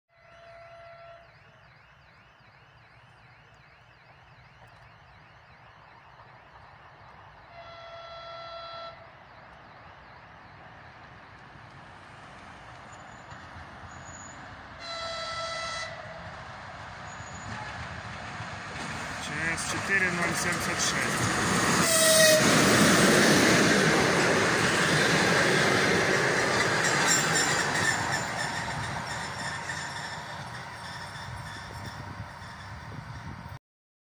"Карантинная Развозка" сменных жд из за отмены пригородных 7608 Тетерев-Киев-Дарница ст.Немешаево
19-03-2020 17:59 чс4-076
 Автор: